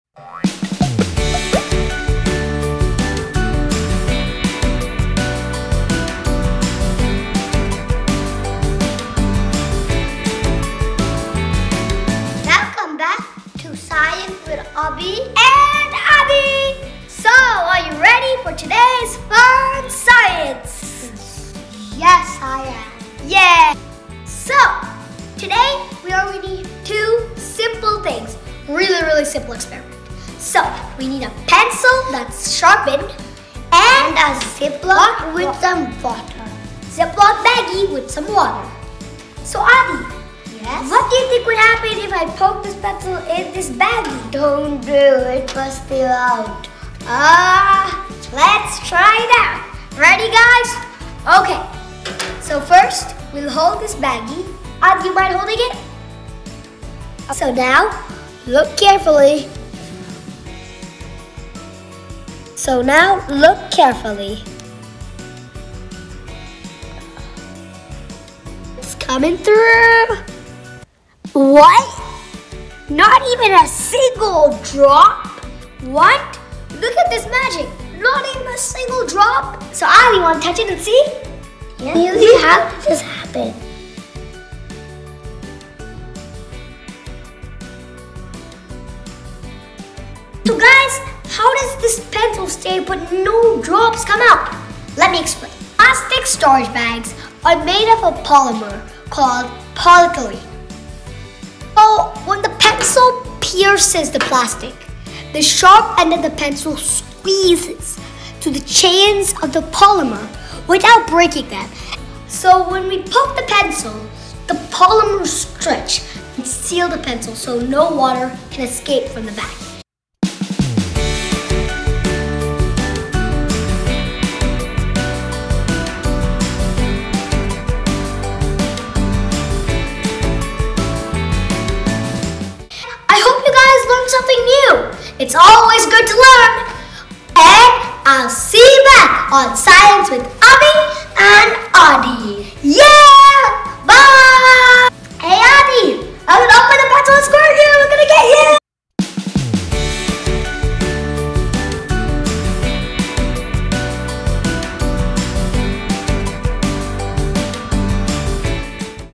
Science for kids | Will the water leak when you poke the pencil through a plastic bag of water ??
What happens when you poke a pencil through a plastic bag filled with water ?? Will the water leak ?? Let us do a fun science experiment to find it out.